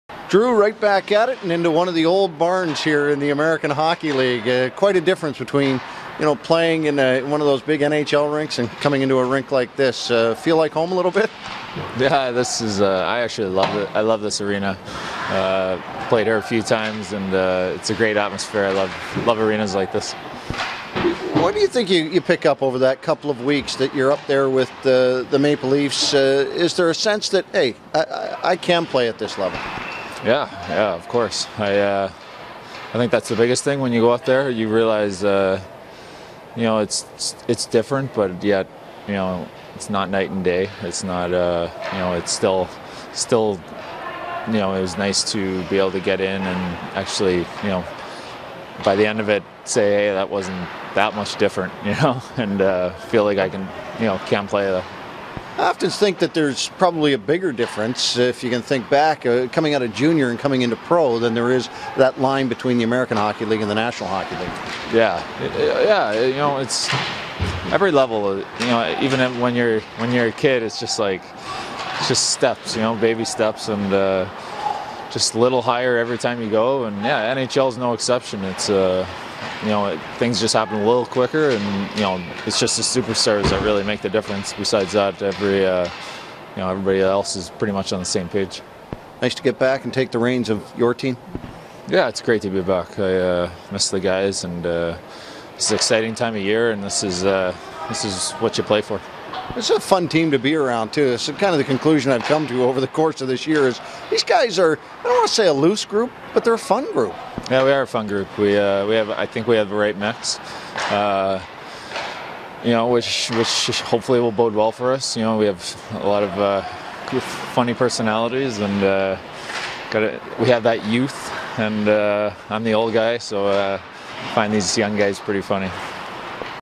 Drew MacIntyre - 04/17/2014
Drew MacIntyre speaks with Todd Crocker after pactice on Thursday.